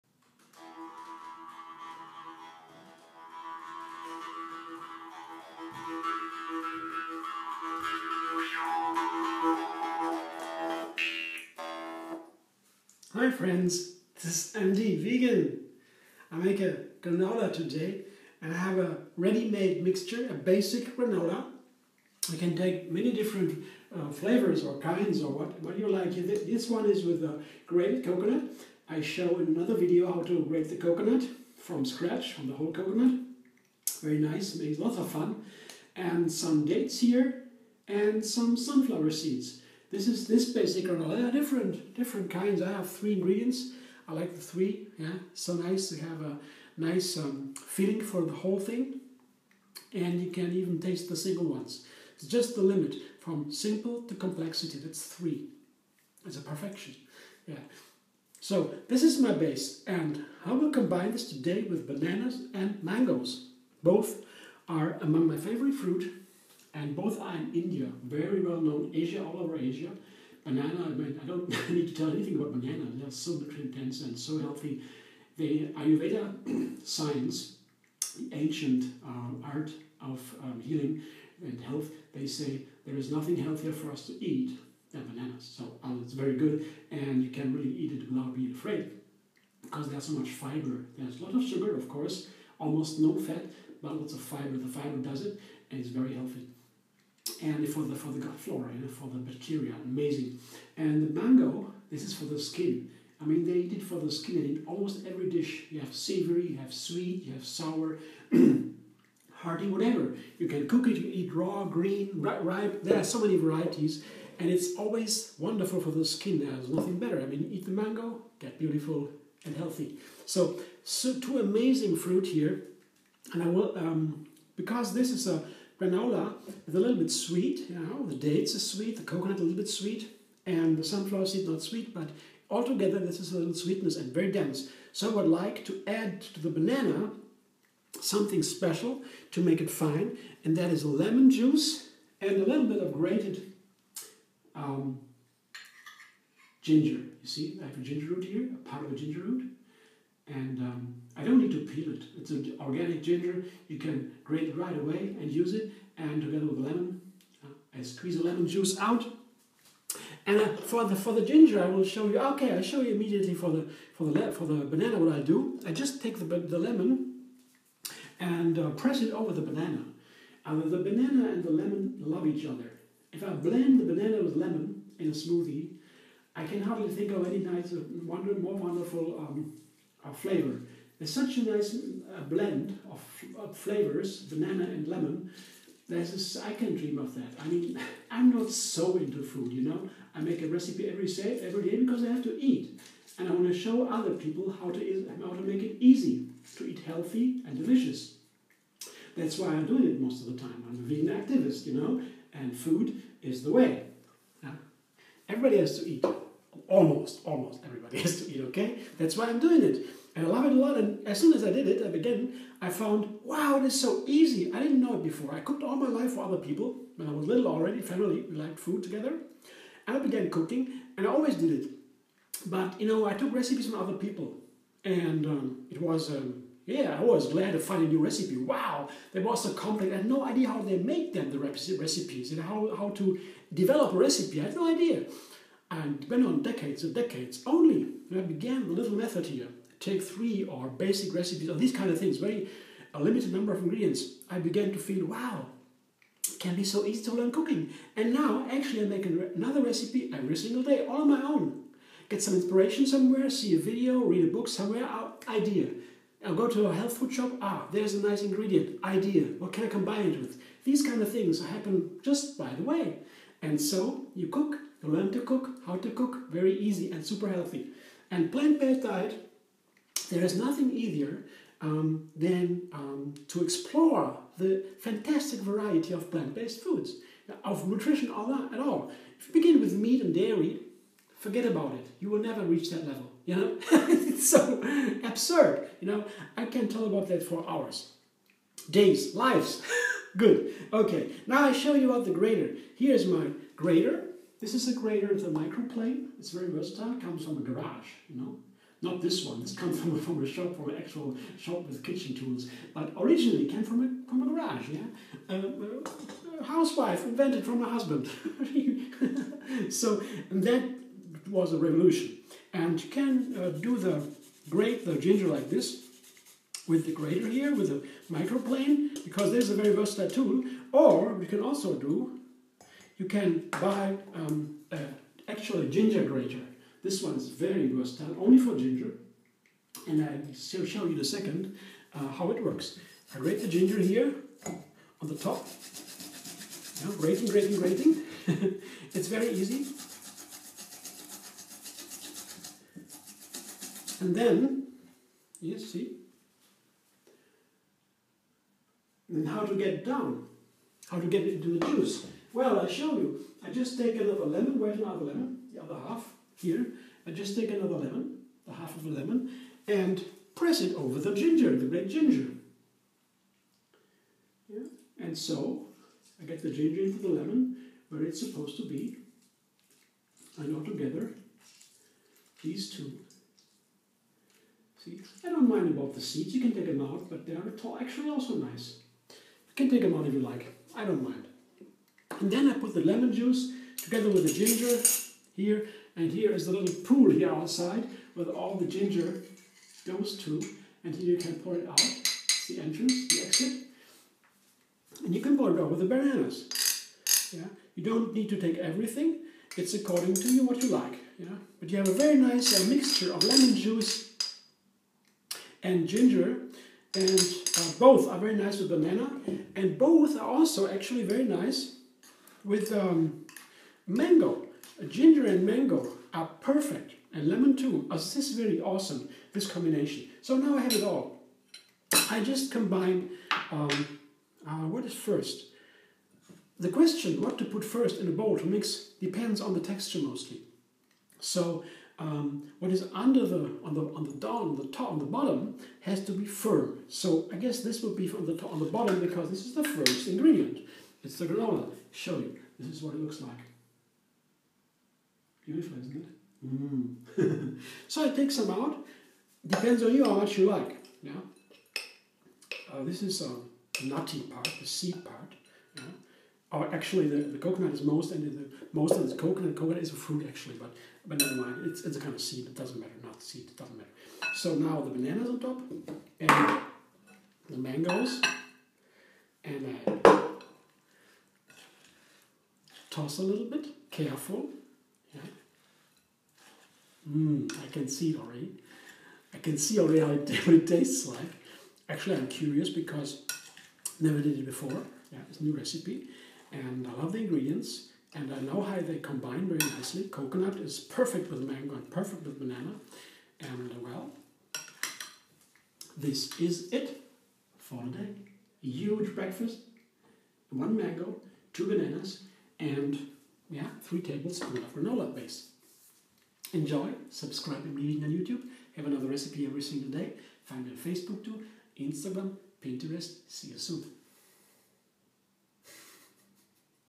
Banana Mango Granola ~ Take 3
MD Vegan refining bananas with lemon and ginger, combining with mango, combining with Basic Granola.